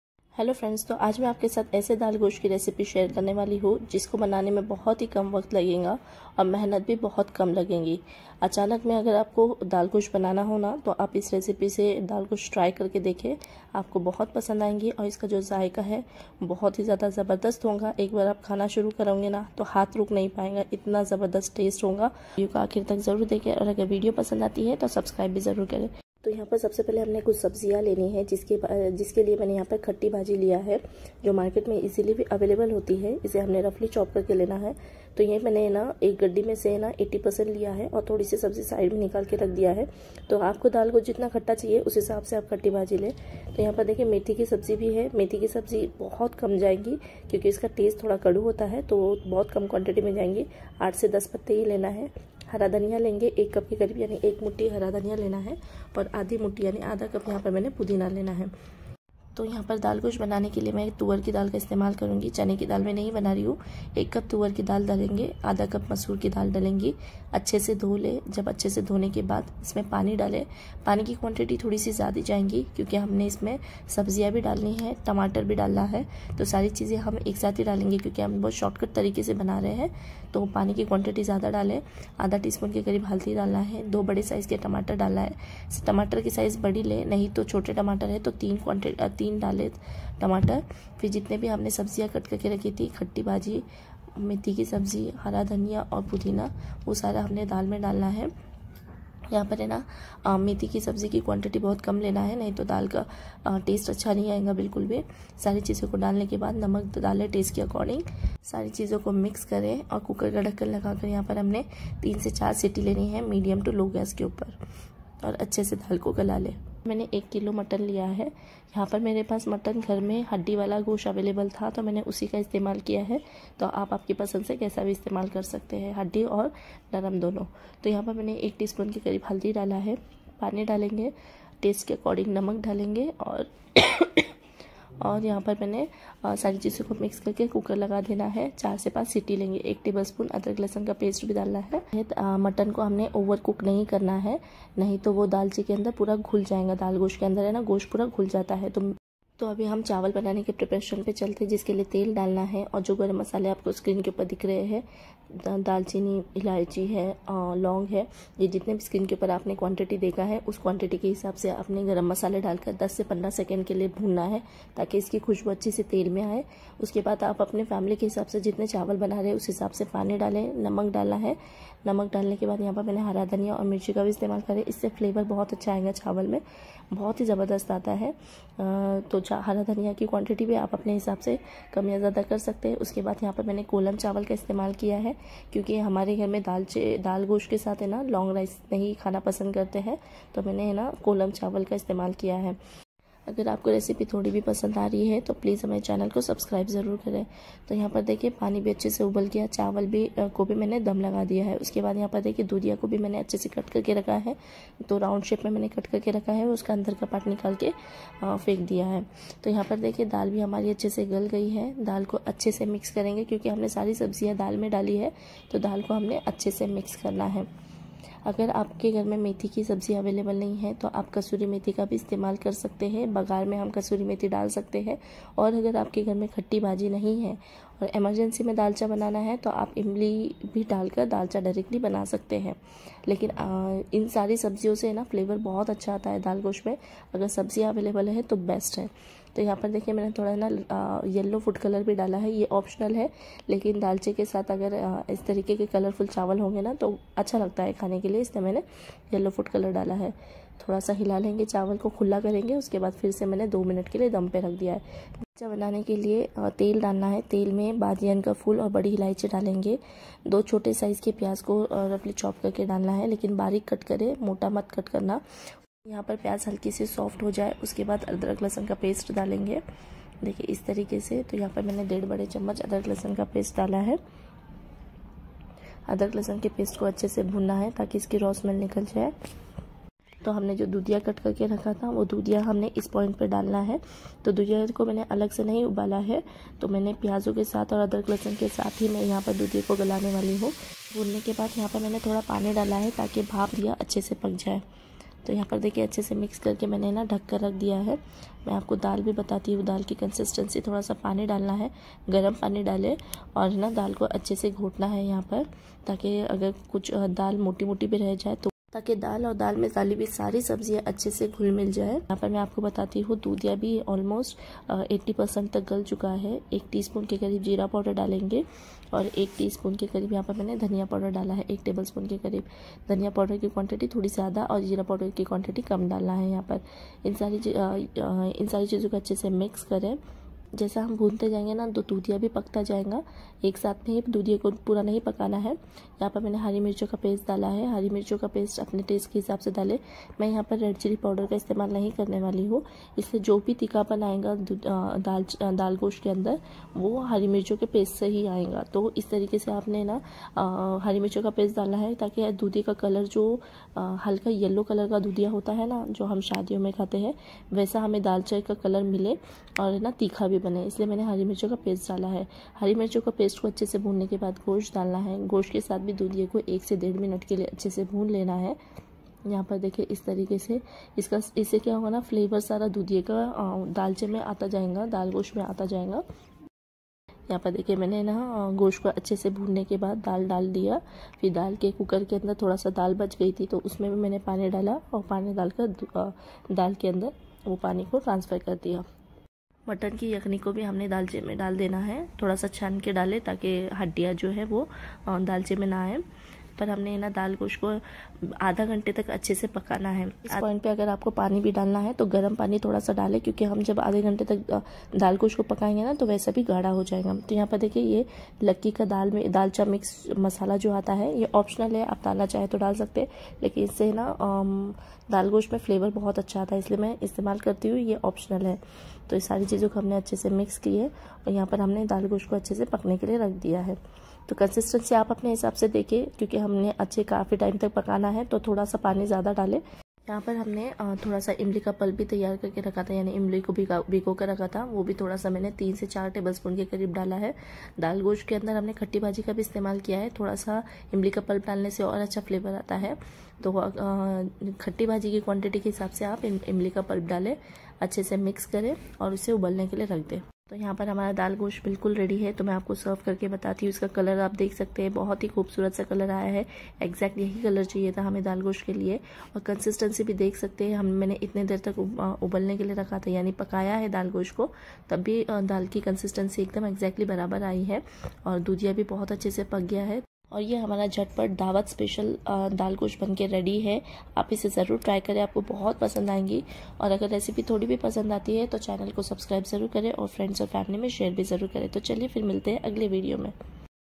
Jhatpat wala Mutton Daal Gosht Bagara Rice | Daal Gosht Recipe Step by Step| Daal Gosht kaise banaye
Hie Friends.
Jhatpat Dalgosht Banaye aur Dawat ko Special Banaye . Kam waqt me kam mehnat me zabardast daal gosht banaye .

New  Uploaded Videos.

Related keywords.